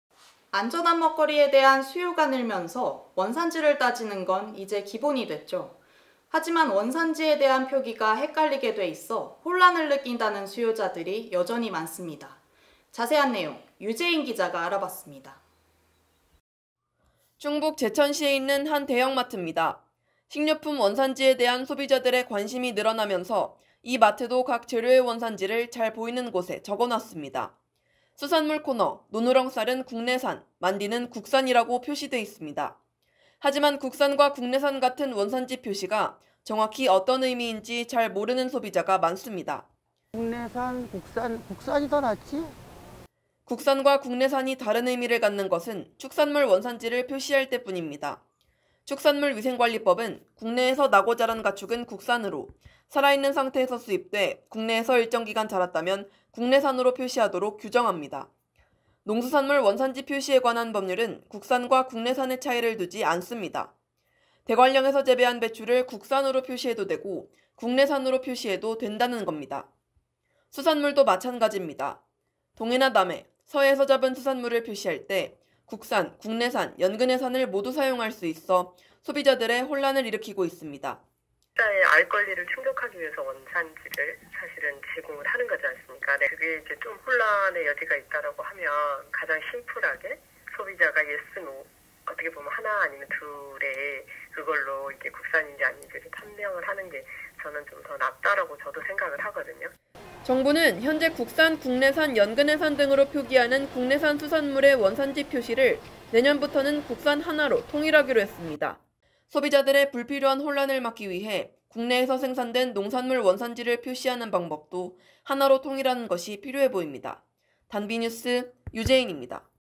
[단비리포트] 국산일까 국내산일까? 헷갈리는 원산지 표시
[앵커]

안전한 먹거리에 대한 수요가 늘면서 원산지를 따지는 건 이제 기본이 됐죠.

하지만 원산지 표기가 헷갈리게 돼 있어 혼란을 느낀다는 수요자들이 여전히 많습니다.

자세한 내용, 유재인 기자가 알아봤습니다.

[리포트]

충북 제천시에 있는 한 대형마트입니다.

식료품 원산지에 대한 소비자들의 관심이 늘어나면서, 이 마트도 각 재료의 원산지를 잘 보이는 곳에 적어 놨습니다.

수산물 코너, 논우렁살은 ‘국내산’, 만디(미더덕)는 ‘국산’ 이라고 표시돼 있습니다.

하지만 국산과 국내산 같은 원산지 표시가 정확히 어떤 의미인지 잘 모르는 소비자가 많습니다.

[강산구 / 제천 시민 : 국내산 국산.. (중에는) 국산이 낫지.]

‘국산’과 ‘국내산’이 다른 의미를 갖는 것은 축산물 원산지를 표기할 때뿐입니다.

축산물위생관리법은 국내에서 나고 자란 가축은 ‘국산’으로, 살아있는 상태에서 수입돼 국내에서 일정 기간 자랐다면 ‘국내산’으로 표시하도록 규정합니다.

농수산물 원산지 표시에 관한 법률은 ‘국산’과 ‘국내산’의 차이를 두지 않습니다.

대관령에서 재배한 배추를 ‘국산’으로 표시해도 되고 ‘국내산’으로 표시해도 된다는 겁니다.

수산물도 마찬가집니다.

동해나 남해, 서해에서 잡은 수산물을 표시할 때 국산, 국내산, 연근해산을 모두 사용할 수 있어 소비자들의 혼란을 일으키고 있습니다.

[백은영 / 한국해양수산개발원 연구원 : 소비자의 알 권리를 충족하기 위해 원산지를 제공하는 것이지 않습니까. (중략) 근데 그게 혼란의 여지가 있다고 하면 가장 심플하게 소비자가 예스 노, 하나 아니면 둘로 국산인지 아닌지 판명을 하는 게 좀 더 낫다고 저도 생각을 하거든요.]

정부는 현재 '국산, 국내산, 연근해산' 등으로 표기하는 국내산 수산물의 원산지 표시를 내년부터는 ‘국산’ 하나로 통일하기로 했습니다.

소비자들의 불필요한 혼란을 막기 위해 국내에서 생산된 농산물 원산지를 표시하는 방법도 하나로 통일하는 것이 필요해 보입니다.

단비뉴스 유재인입니다.

(촬영 : 유재인, 김현주 기자 / 편집 : 신현우 PD, 유재인 기자 / 그래픽 : 신현우 PD, 유재인 기자 / 앵커 : 이정민 기자)